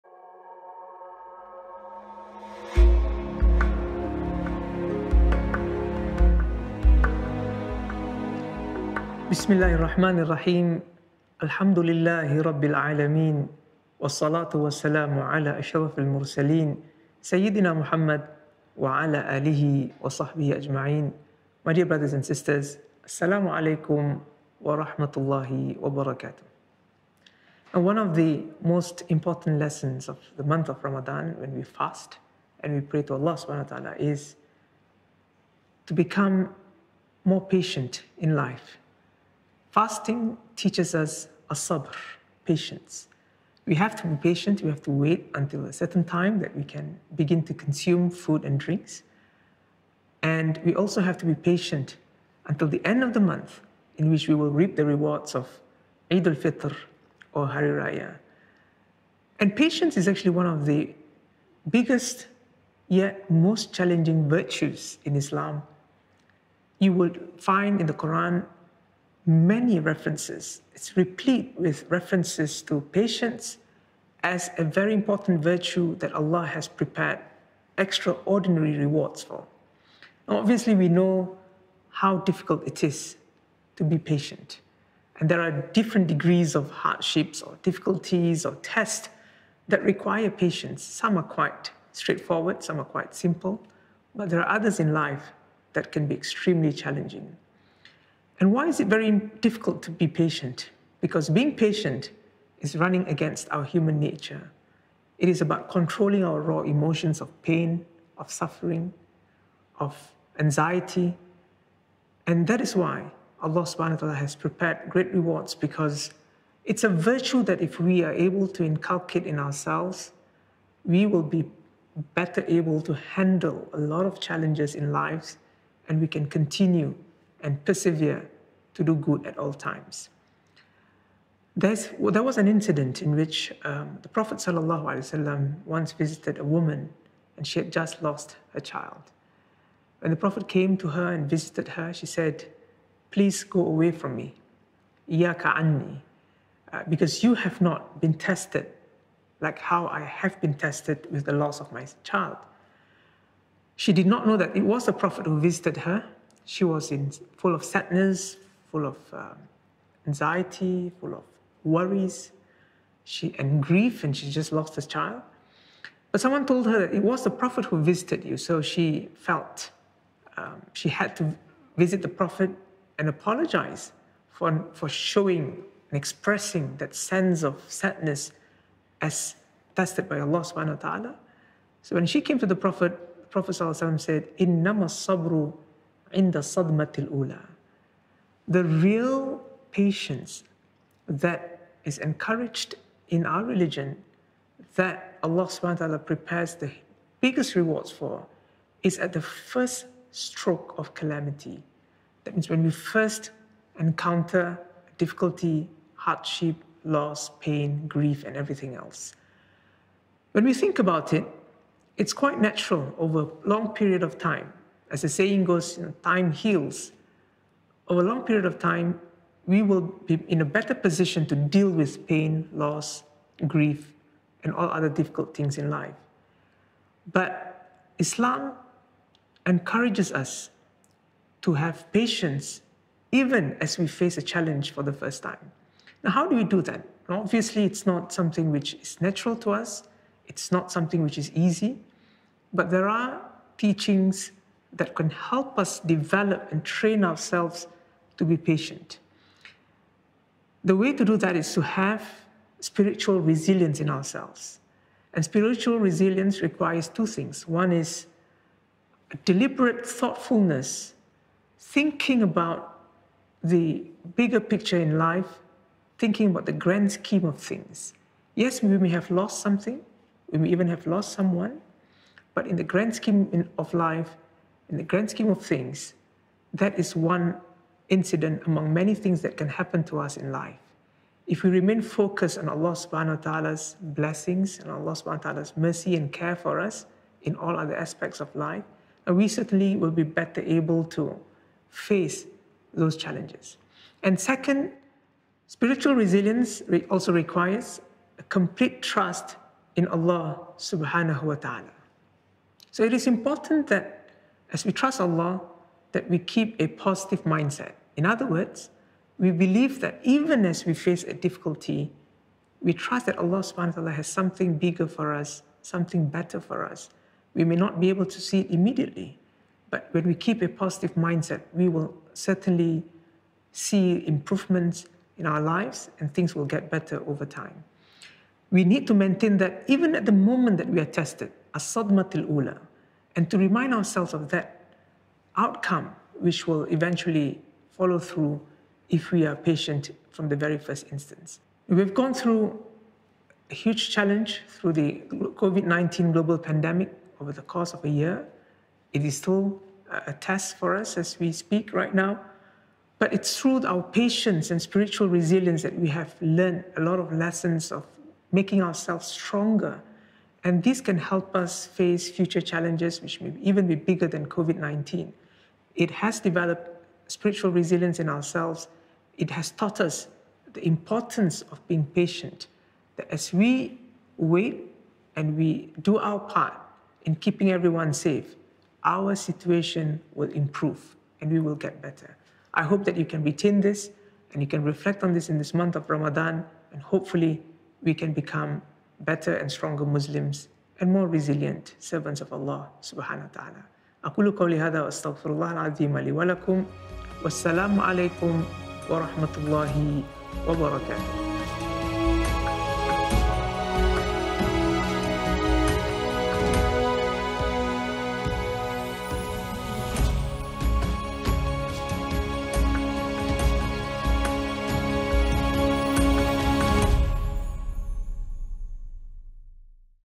How To Be Patient in Islam | Ramadan Letters From Mufti | Dr Nazirudin Mohd Nasir
Prophet s.a.w. said:
إِنَّما الصَّبْرُ عِنْدَ الصَّدْمَةِ الأُولَى

The real patience that is encouraged in our religion that Allah s.w.t. prepares the biggest rewards for is at the first stroke of calamity.

That means when you first encounter difficulty, hardship, loss, pain, grief and everything else. When we think about it, it's quite natural over a long period of time, as the saying goes — time heals — over a long period of time, we will be in a better position to deal with pain, loss, grief and all other difficult things in life.

But Islam encourages us to have patience even as we face a challenge for the first time.